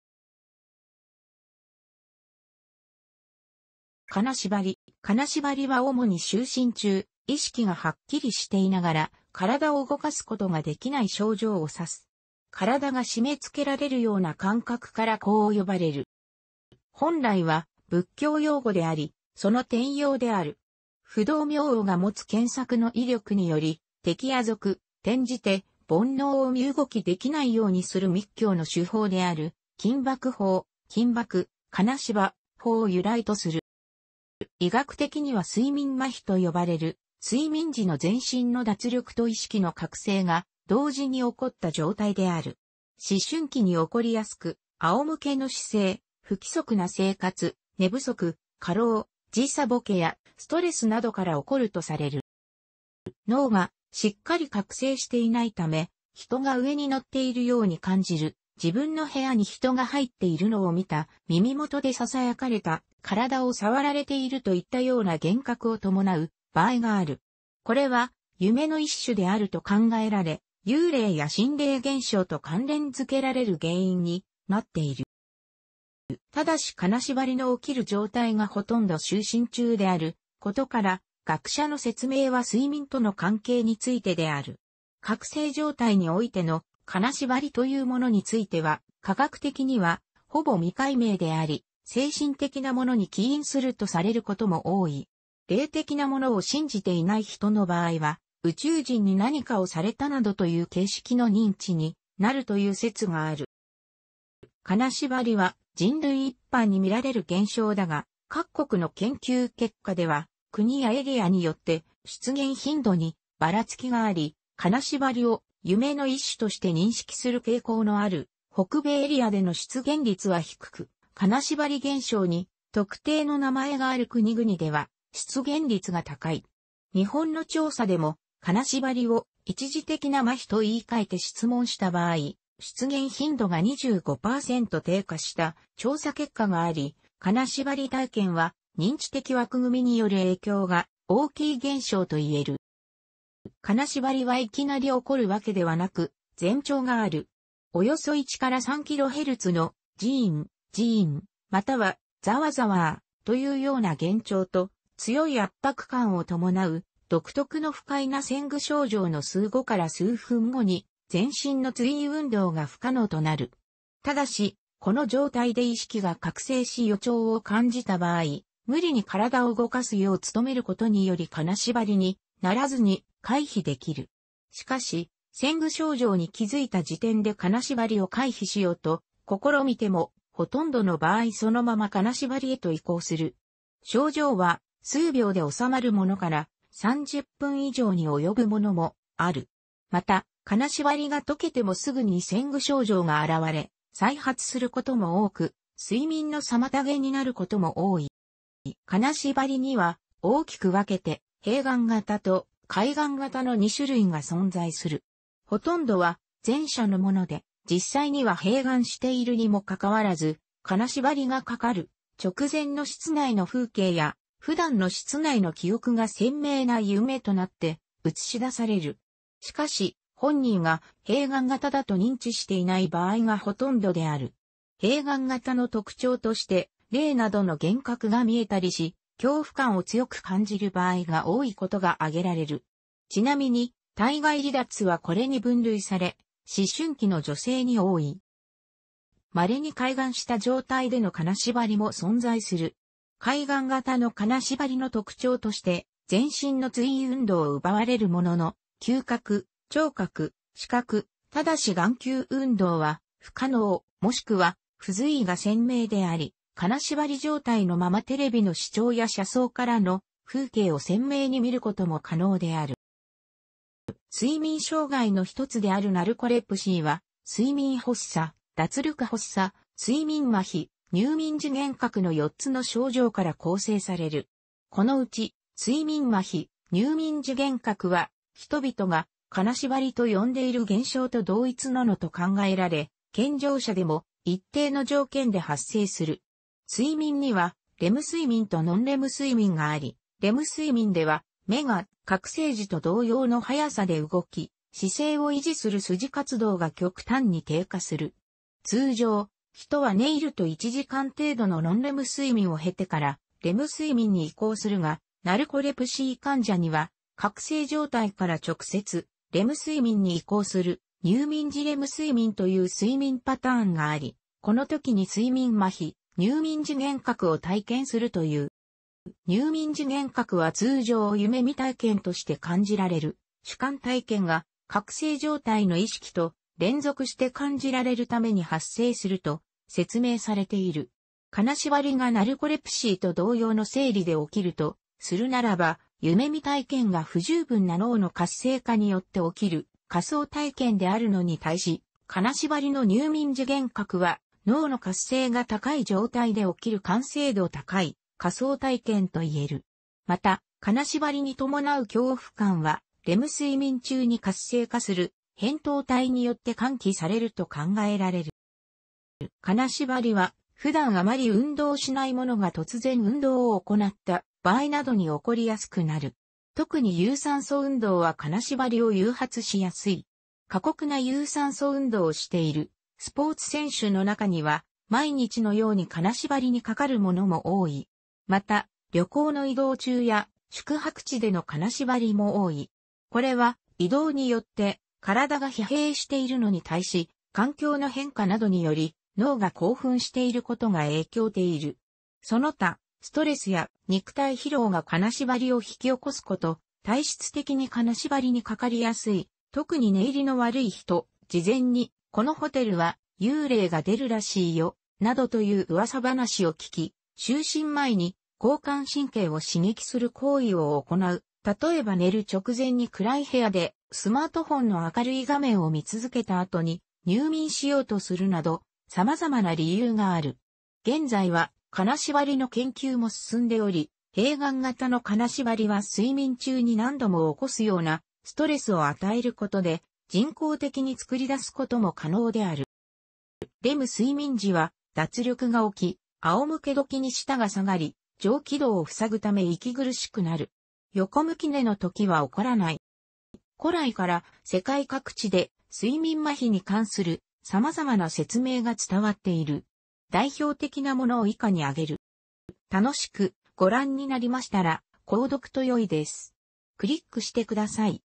金縛り
金縛り（かなしばり）は主に就寝中、意識がはっきりしていながら身体を動かすことができない症状を指す。身体が締め付けられるような感覚からこう呼ばれる。

本来は仏教用語であり、その転用である。不動明王が持つ羂索（けんさく）の威力により、敵や賊（転じて煩悩）を身動きできないようにする密教の修法である「金縛法」（きんばく・かなしばりほう）を由来とする。

医学的には睡眠麻痺と呼ばれる、睡眠時の全身の脱力と意識の覚醒が同時に起こった状態である。思春期に起こりやすく、仰向けの姿勢、不規則な生活、寝不足、過労、時差ぼけやストレスなどから起こるとされる。

脳がしっかり覚醒していないため、人が上に乗っているように感じる、自分の部屋に人が入っているのを見た、耳元で囁かれた、身体を触られているといったような幻覚を伴う場合がある。これは夢の一種であると考えられ、幽霊や心霊現象と関連づけられる原因になっている。

ただし金縛りの起きる状態がほとんど就寝中であることから、学者の説明は睡眠との関係についてである。覚醒状態においての「金縛り」というものについては科学的にはほぼ未解明であり、精神的なものに起因するとされることも多い。霊的なものを信じていない人の場合は、宇宙人に何かをされたなどという形式の認知になるという説がある。

金縛りは人類一般に見られる現象だが、各国の研究結果では国やエリアによって出現頻度にばらつきがあり、金縛りを夢の一種として認識する傾向のある北米エリアでの出現率は低く、金縛り現象に特定の名前がある国々では出現率が高い。日本の調査でも「金縛り」を「一時的な麻痺」と言い換えて質問した場合、出現頻度が25パーセント低下した調査結果があり、金縛り体験は認知的枠組みによる影響が大きい現象と言える。

金縛りはいきなり起こるわけではなく、前兆がある。およそ1～3キロヘルツ (kHz) の「ジーン、ジーン」または「ザワザワー」というような幻聴と、強い圧迫感を伴う独特の不快な前駆症状の数秒後～数分後に、全身の随意運動が不可能となる。但し、この状態で意識が覚醒し予兆を感じた場合、無理に身体を動かすよう努める事により金縛りにならずに回避できる。しかし、前駆症状に気づいた時点で金縛りを回避しようと試みても、ほとんどの場合そのまま金縛りへと移行する。症状は数秒で収まるものから、30分以上に及ぶものもある。また、金縛りが解けてもすぐに前駆症状が現れ、再発することも多く、睡眠の妨げになることも多い。

金縛りには、大きく分けて、閉眼型と、開眼型の二種類が存在する。ほとんどは前者のもので、実際には閉眼しているにもかかわらず、金縛りがかかる直前の室内の風景や、普段の室内の記憶が鮮明な夢となって映し出される。しかし、本人が閉眼型だと認知していない場合がほとんどである。閉眼型の特徴として、霊などの幻覚が見えたりし、恐怖感を強く感じる場合が多いことが挙げられる。ちなみに、体外離脱はこれに分類され、思春期の女性に多い。

まれに開眼した状態での金縛りも存在する。開眼型の金縛りの特徴として、全身の随意運動を奪われるものの、嗅覚、聴覚、視覚（ただし眼球運動は不可能(可能な場合もある)、もしくは不随意）が鮮明であり、金縛り状態のままテレビの視聴や車窓からの風景を鮮明に見ることも可能である。

睡眠障害のひとつであるナルコレプシーは、睡眠発作、脱力発作、睡眠麻痺、入眠時幻覚の4つの症状から構成される。このうち、睡眠麻痺、入眠時幻覚は人々が金縛りと呼んでいる現象と同一のものと考えられ、健常者でも一定の条件で発生する。睡眠にはレム睡眠とノンレム睡眠があり、レム睡眠では目が覚醒時と同様の早さで動き、姿勢を維持する筋活動が極端に低下する。通常、人は寝入ると1時間程度のノンレム睡眠を経てからレム睡眠に移行するが、ナルコレプシー患者には覚醒状態から直接レム睡眠に移行する入眠時レム睡眠という睡眠パターンがあり、この時に睡眠麻痺、入眠時幻覚を体験するという。

入眠時幻覚は通常夢見体験として感じられる主観体験が、覚醒状態の意識と連続して感じられるために発生すると説明されている。金縛りがナルコレプシーと同様の生理でおきるとするならば、夢見体験が不十分な脳の活性化によって起きる仮想体験であるのに対し、金縛りの入眠時幻覚は脳の活性が高い状態で起きる完成度の高い仮想体験と言える。また、金縛りに伴う恐怖感は、レム睡眠中に活性化する扁桃体によって喚起されると考えられる。

金縛りは、普段余り運動しない者が突然運...